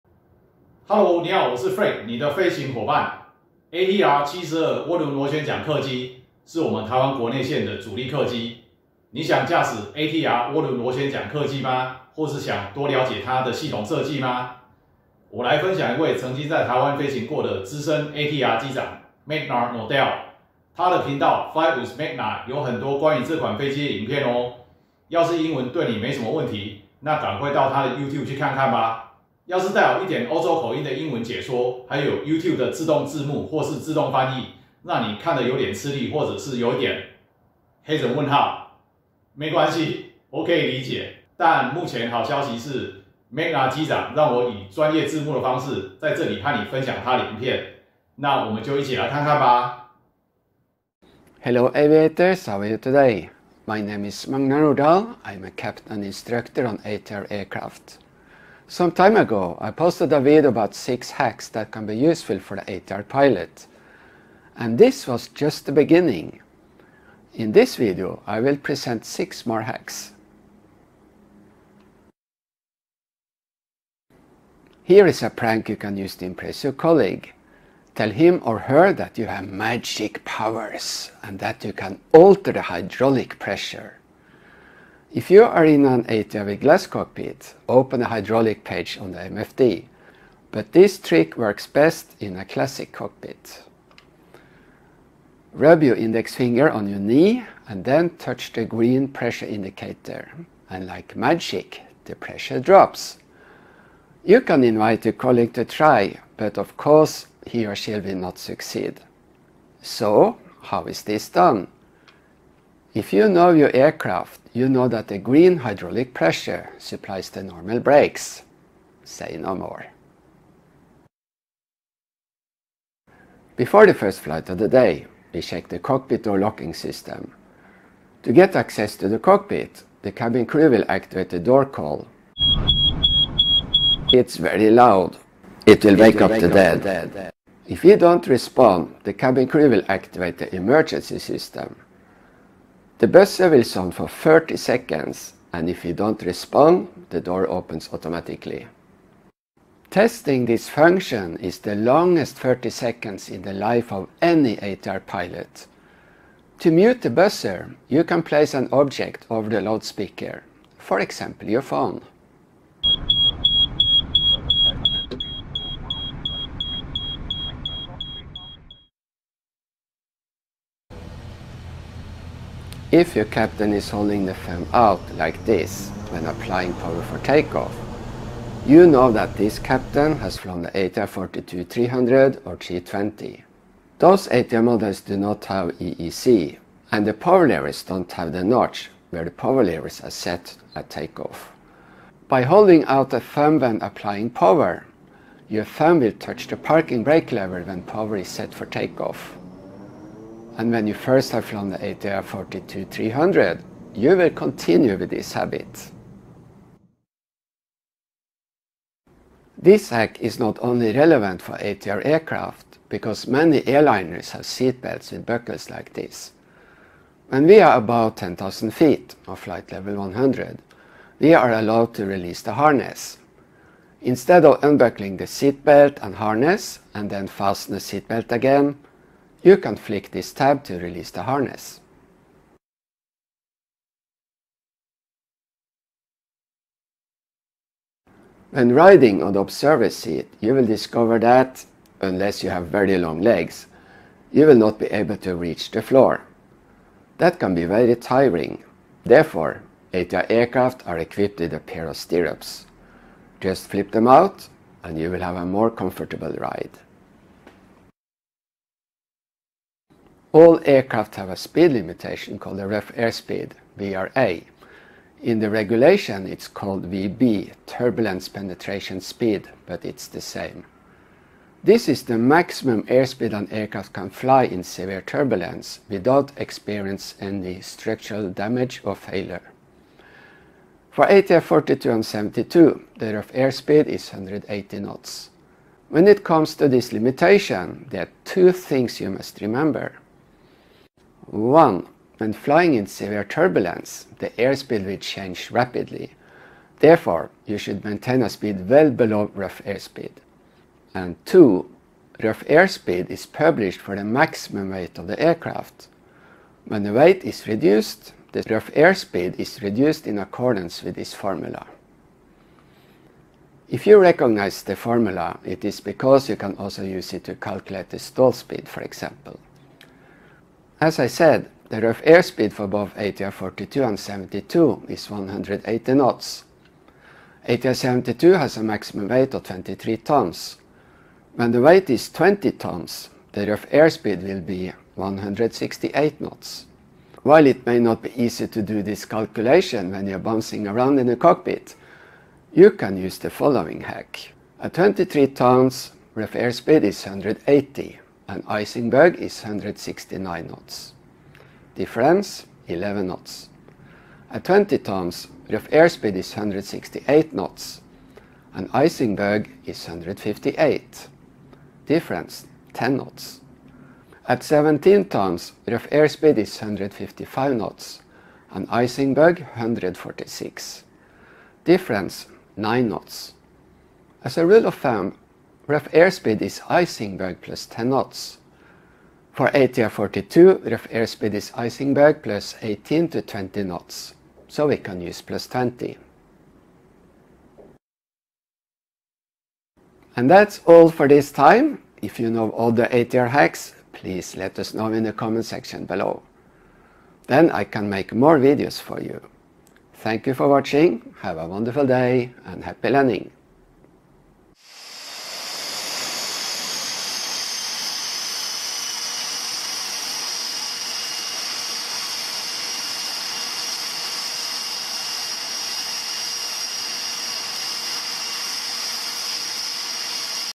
ATR 72 你可能不知道的密技 PART 2
ATR 72 你可能不知道的密技 PART 2. 在這集裡, Magnar 機長幫你講解 ATR-72 飛機一些只有內行人才知道的小撇步, 趕快點進來看看吧! 記得也到Fly With Magnar 機長的頻道去看看喔.